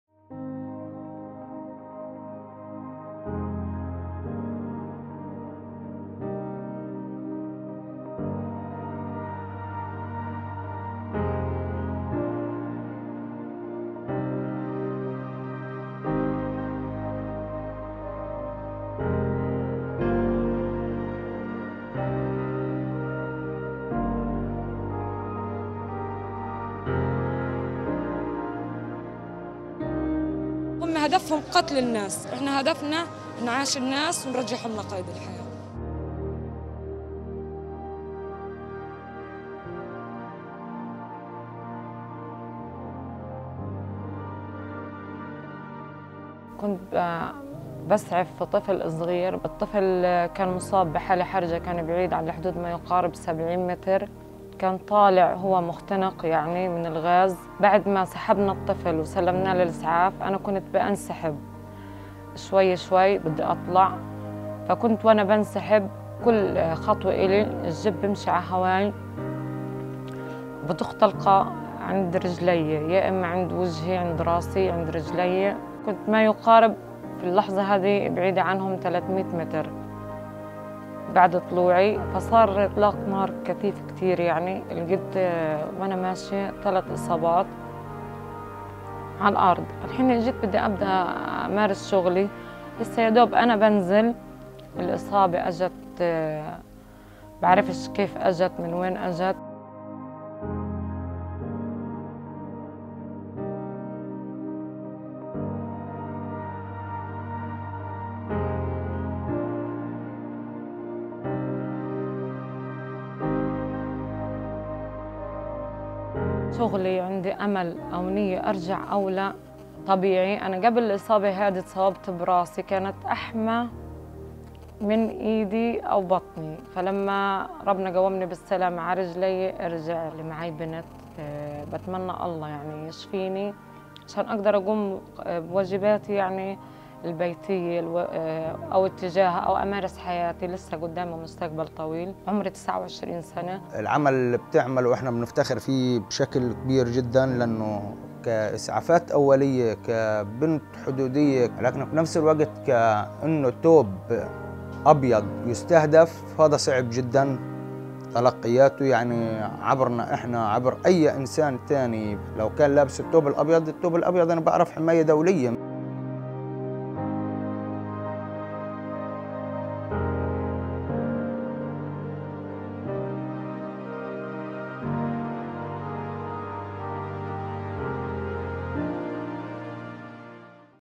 Gaza Paramedic Sabreen shot a second time
Gaza paramedic Sabreen Qeshta describes in MAP’s new film being shot while treating injured demonstrators at the “Great March of Return” and the impact this has had on her life.